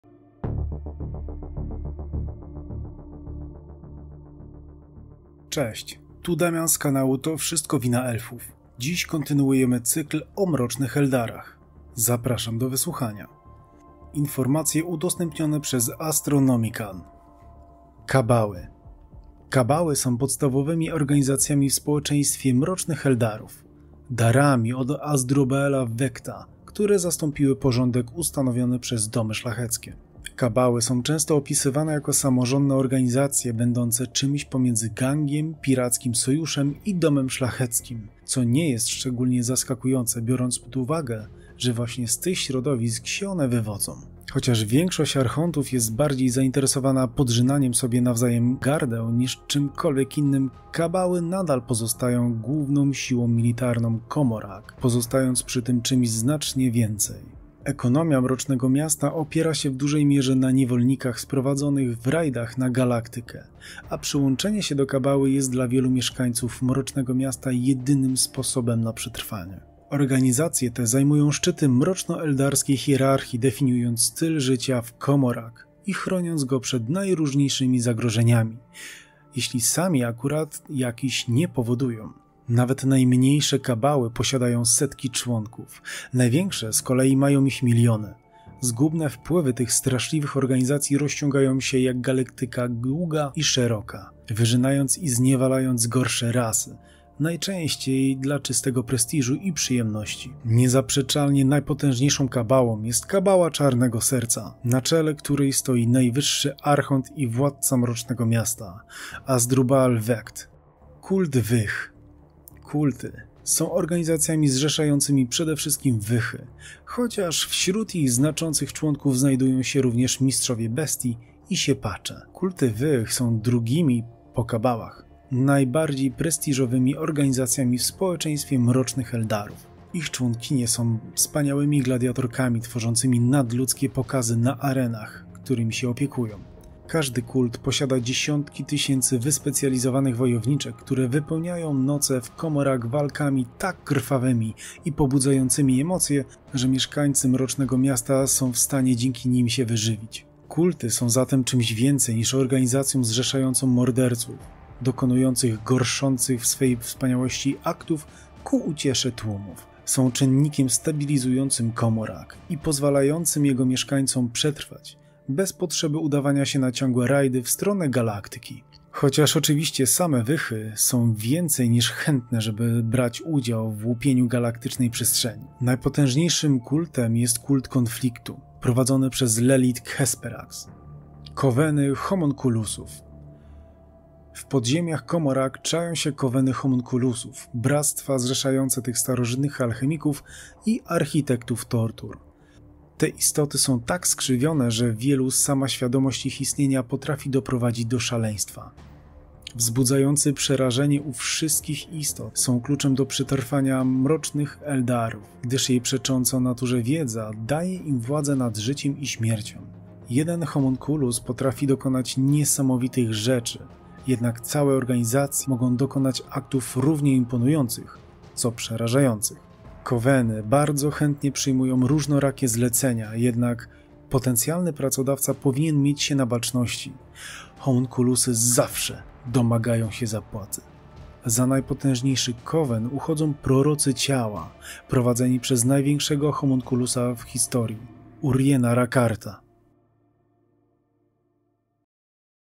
Warhammer 40 000 - Wrath and Glory - Drukhari cz. 4 - Mroczni Eldarzy - Kabały
Warhammer 40 000 - Wrath and Glory
Drukhari - Mroczni Eldarzy - Kabały

Czyta: Damian @demonprzymikrofonie-qy6fx
Grafika: Wojtek

Chcesz wspomóc naszą działalność? Zapraszamy na Patronait

Informacje pochodzą z astronomicanu